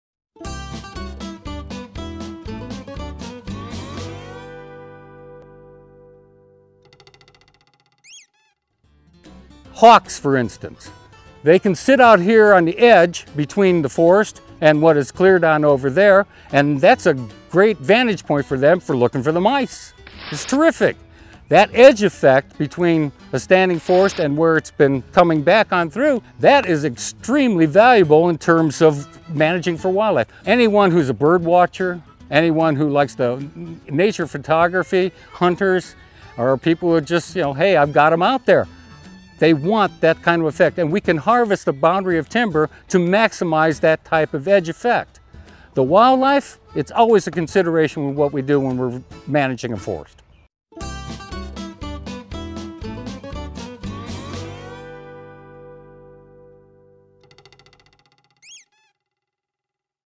What is edge effect?
Roy Jarrard, forestry expert, explains Edge Effect.